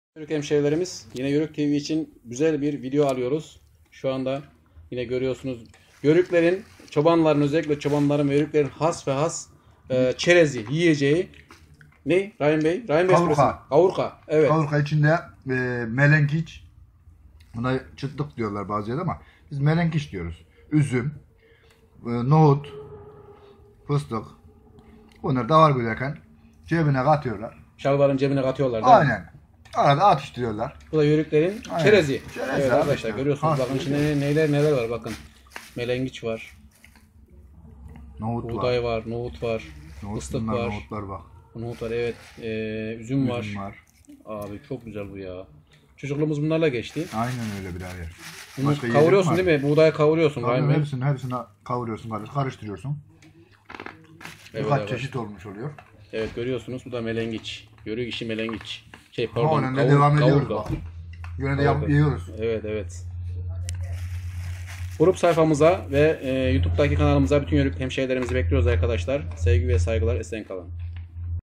KAVURGA NEDİR,MELENGİÇ NEDİR,YÖRÜK İŞİ KAVURKA NASIL YAPILIR,YÖRÜK TV DE ,YÖRÜK VİDEOLARI
Yörüklerimizin vaz geçilmez çıtlığı,eğlencelik ve aparatif yiyeceği çıtlık,kavurga,kavurga.Genelde Misafir geldiğinde hazırlanırdı eskiden.Ayrıca Davar ,Koyun,Büyük baş hayvan güderken ya bir keseye yada şalvarın cebine konulur yenirdi. Hey gidi günler hey...